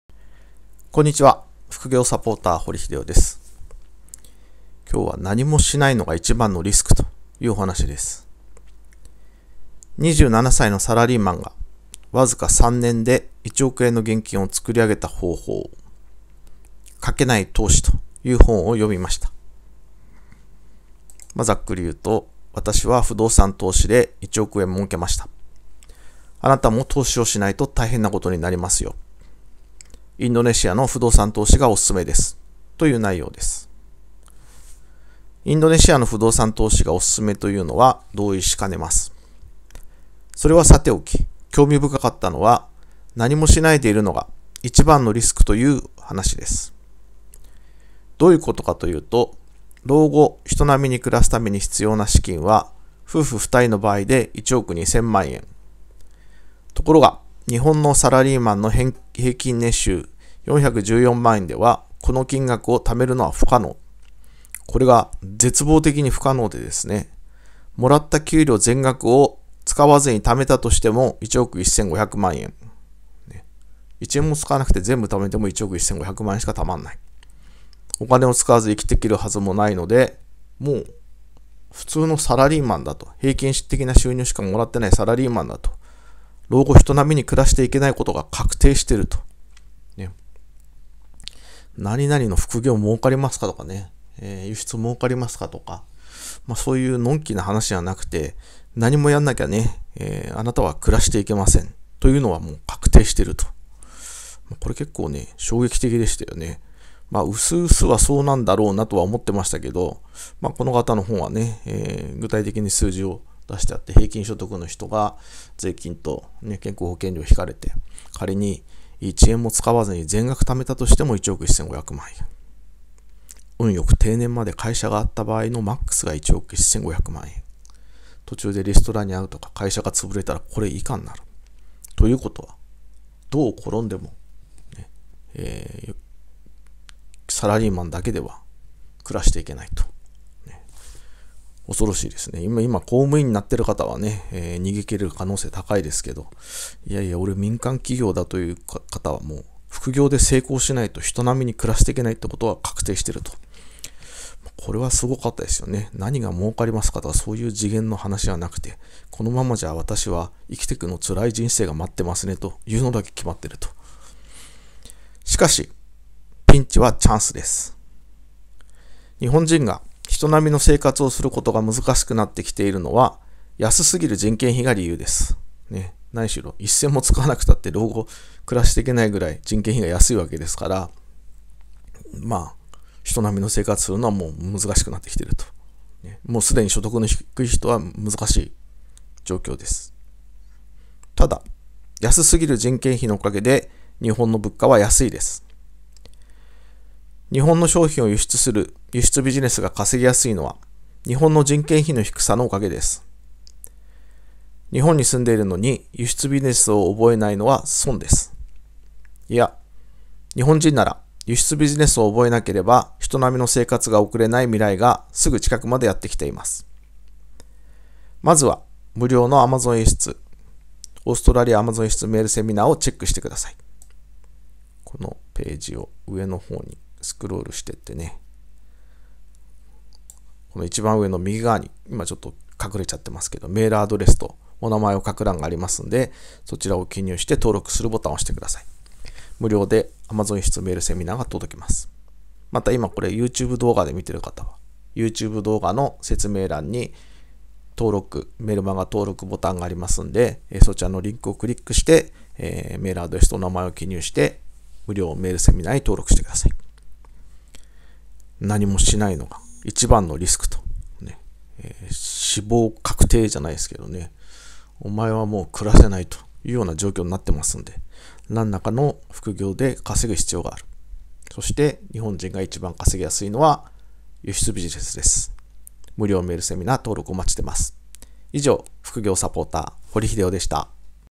給料収入だけの人は老後人並みに暮らしていけないことが確定
賭けない投資レビュー私は中邨 宏季です。不動産投資で1億円儲けました。あなたも投資をしないと大変なことになりますよ。インドネシアの不動産投資がお勧めです。と言う内容です。

1日10分で月10万円を稼ぐ! Amazon輸出ビジネスがわかる本(2013/11/22)